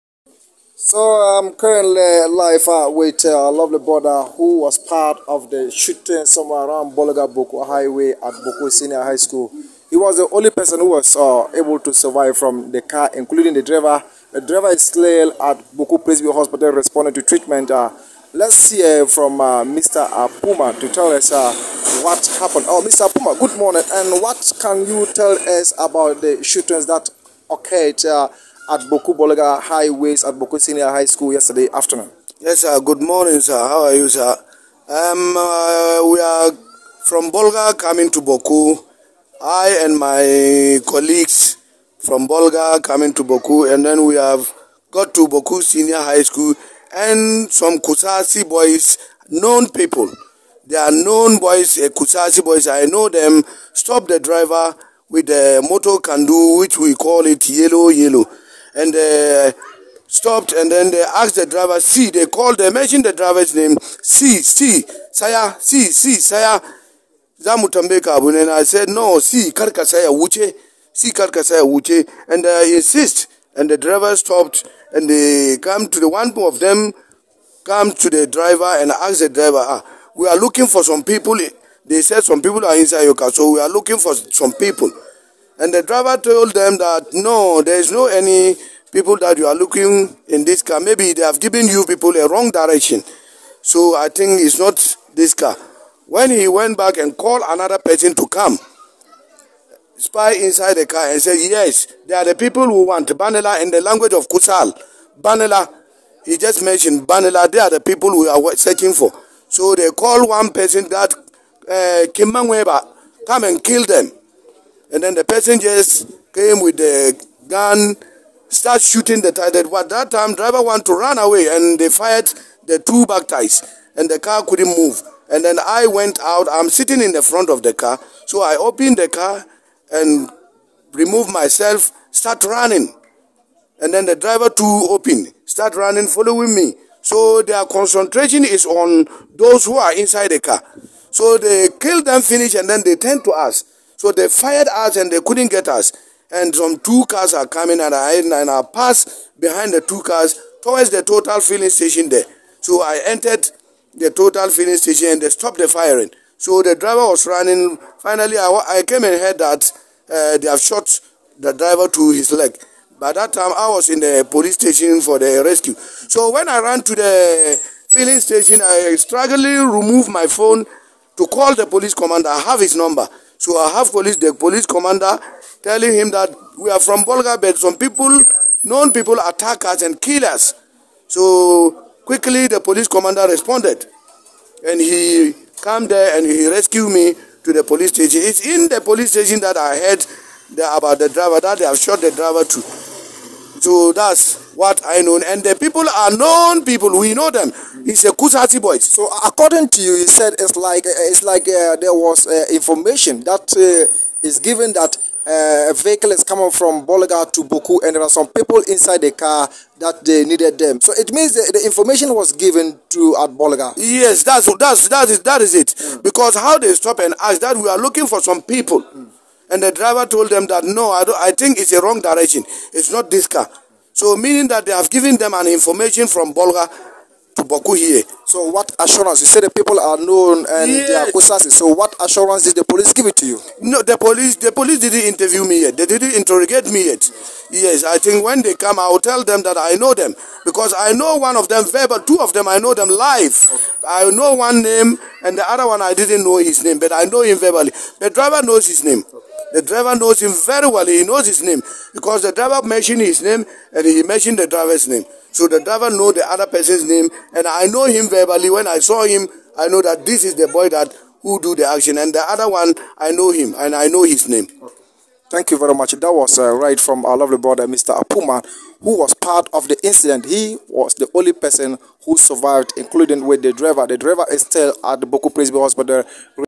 Bawku | DJ WOYOME Interview With An Eye Witness To The Recent Attack On The Bolga-Bawku Highway Road
An Eye Witness Narrative On The Recent Attack By Known Kusasis Bandits Who Shot & Killed 3 Persons On The Bolga-Bawku Highway Road (Entrance Of Bawku Senior High School) On Tuesday 16-August-2022 Around 1:30pm.